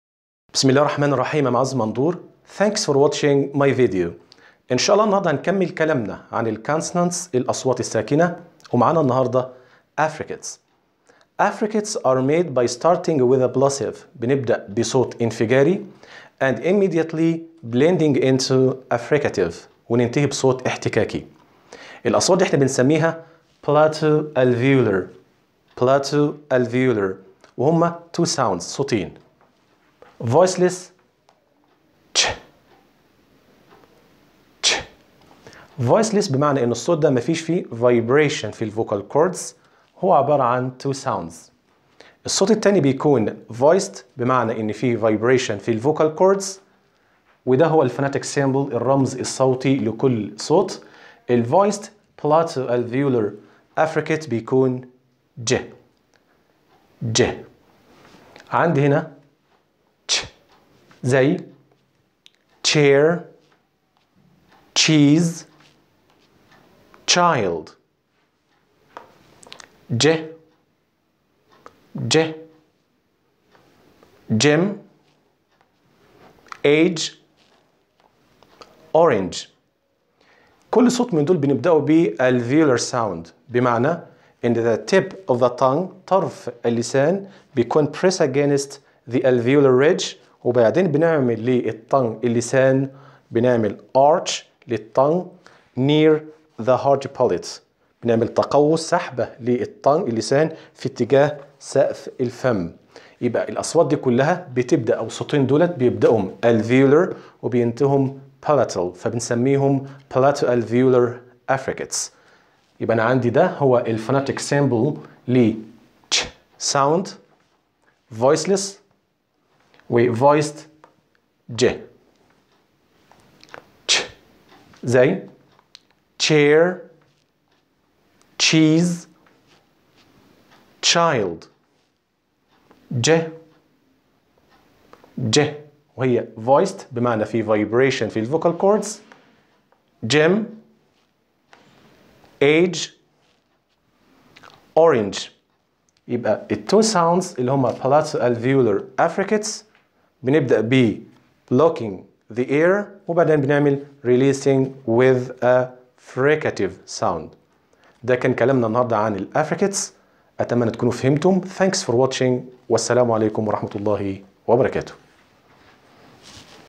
English Consonants_ affricates: /ʧ/ and /ʤ/
The English affricates, the  'ch sound' /ʧ/ and 'j sound' /ʤ/ are two-part consonant sounds. They begin by fully stopping the air from leaving the vocal tract (similar to a stop sound), then releasing it through a constricted opening. (similar to a fricative sound).